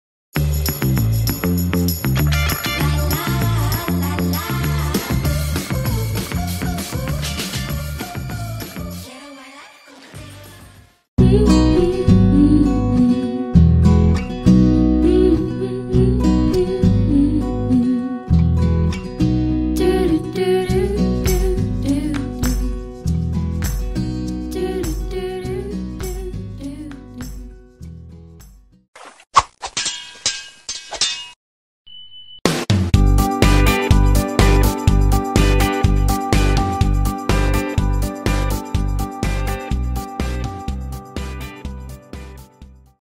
Herbal Tea Recipe's Intro - Best Winter Drink - Magical Drink with Instant Relief
Relieve stress, boost your immunity and get ready for the day as you sip on this magical herbal drink